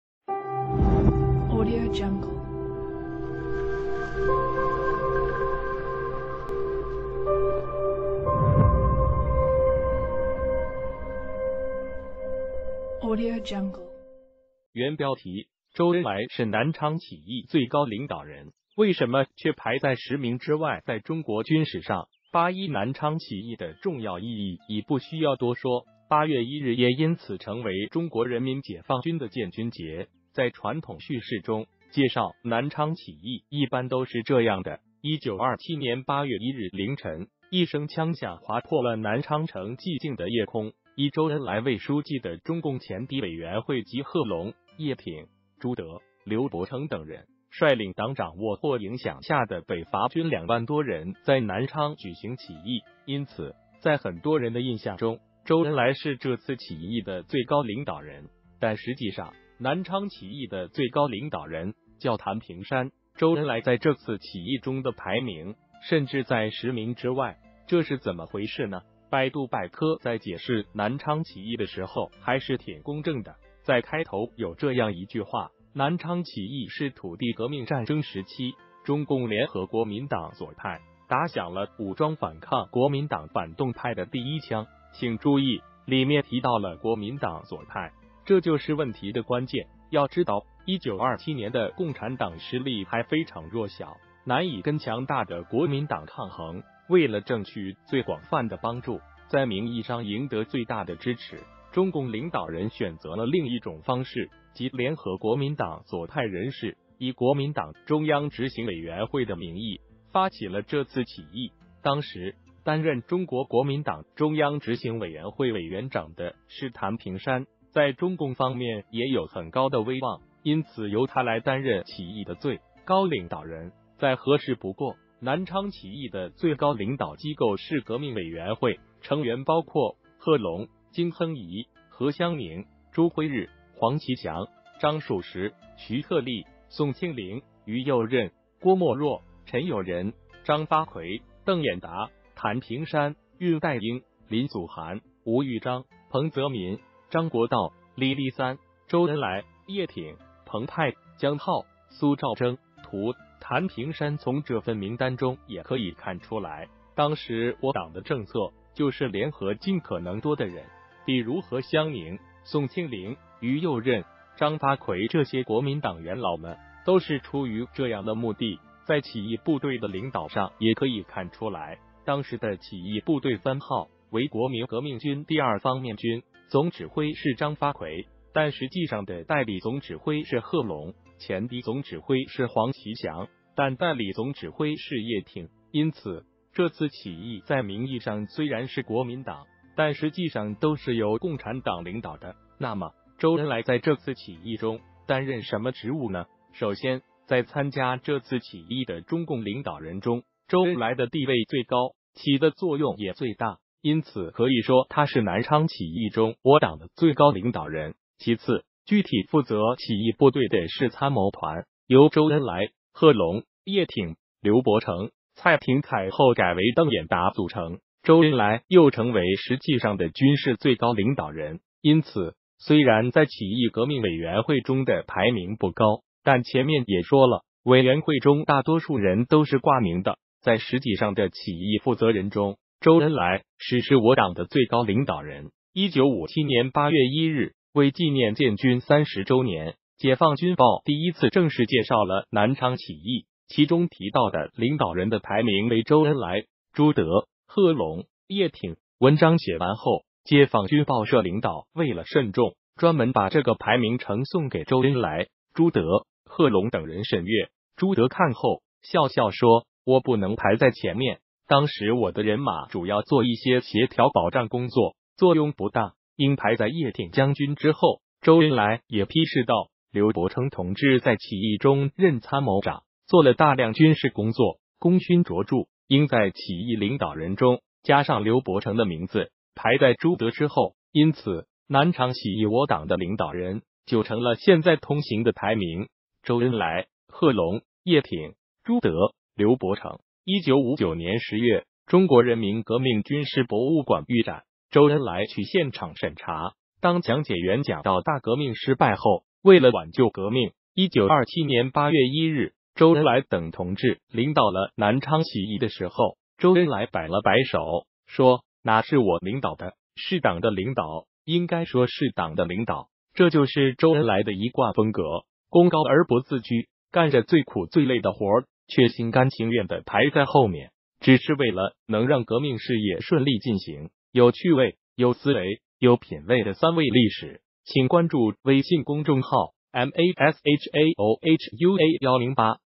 周恩来是南昌起义最高领导人，为什么却排在十名之外
原标题：周恩来是南昌起义最高领导人，为什么却排在十名之外在中国军史上，八一南昌起义的重要意义已不需要多说，八月一日也因此成为中国人民解放军的建军节。在传统叙事中，介绍南昌起义一般都是这样的：“1927年8月1日凌晨，一声枪响划破了南昌城寂静的夜空。以周恩来为书记的中共前敌委员会及贺龙、叶挺、朱德、刘伯承等人，率领党掌握或影响下的北伐军两万多人在南昌举行起义……”因此，在很多人的印象中，周恩来是这次起义的最高领导人。但实际上，南昌起义的最高领导人叫谭平山，周恩来在这次起义中的排名甚至在十名之外。这是怎么回事呢？百度百科在解释南昌起义的时候还是挺公正的，在开头有这样一句话：“南昌起义是土地革命战争时期，中共联合国民党左派，打响了武装反抗国民党反动派的第一枪。”请注意，里面提到了“国民党左派”，这就是问题的关键。要知道，1927年的共产党实力还非常弱小，难以跟强大的国民党抗衡，为了争取最广泛的帮助，在名义上赢得最大的支持，中共领导人选择了另一种方式，即联合国民党左派人士，以国民党中央执行委员会的名义，发起了这次起义。当时，担任中国国民党中央执行委员会委员长的是谭平山，在中共方面也有很高的威望，因此由他来担任起义的最高领导人，再合适不过。南昌起义的最高领导机构是革命委员会，成员包括：贺龙、经亨颐、何香凝、朱晖日、黄琪翔、张曙时、徐特立、宋庆龄、于右任、郭沫若、陈友仁、张发奎、邓演达、谭平山、恽代英、林祖涵、吴玉章、彭泽民、张国焘、李立三、周恩来、叶挺、澎湃、江浩、苏兆征。（图：谭平山）从这份名单中也可以看出来，当时我党的政策就是联合尽可能多的人，比如何香凝、宋庆龄、于右任、张发奎这些国民党元老们，都是出于这样的目的。在起义部队的领导上也可以看出来，当时的起义部队番号为国民革命军第二方面军，总指挥是张发奎，但实际上的代理总指挥是贺龙；前敌总指挥是黄琪翔，但代理总指挥是叶挺。因此，这次起义在名义上虽然是国民党，但实际上都是由共产党领导的。那么，周恩来在这次起义中担任什么职务呢？首先，在参加这次起义的中共领导人中，周恩来的地位最高，起的作用也最大，因此可以说他是南昌起义中我党的最高领导人。其次，具体负责起义部队的是参谋团，由周恩来、贺龙、叶挺、刘伯承、蔡廷锴（后改为邓演达）组成，周恩来又成为实际上的军事最高领导人。因此，虽然在起义革命委员会中的排名不高，但前面也说了，委员会中大多数人都是挂名的，在实际上的起义负责人中，周恩来确实是我党的最高领导人。1957年8月1日，为纪念建军30周年，《解放军报》第一次正式介绍了南昌起义，其中提到的领导人的排名为周恩来、朱德、贺龙、叶挺。文章写完后，解放军报社领导为了慎重，专门把这个排名呈送给周恩来、朱德、贺龙等人审阅。朱德看后，笑笑说：“我不能排在前面，当时我的人马主要做一些协调保障工作，作用不大，应排在叶挺将军之后。”周恩来也批示道：“刘伯承同志在起义中任参谋长，做了大量军事工作，功勋卓著，应在起义领导人中加上刘伯承的名字，排在朱德之后。”因此，南昌起义我党的领导人就成了现在通行的排名：周恩来、贺龙、叶挺、朱德、刘伯承。1959年10月，中国人民革命军事博物馆预展，周恩来去现场审查，当讲解员讲到“大革命失败后，为了挽救革命，1927年8月1日，周恩来等同志领导了南昌起义”的时候，周恩来摆了摆手，说：“哪是我领导的？是党的领导，应该说是党的领导！”这就是周恩来的一贯风格，功高而不自居，干着最苦最累的活儿，却心甘情愿地排在后面，只是为了能让革命事业顺利进行。有趣味、有思维、有品位的“三味”历史，请关注微信公众号：mashaohua108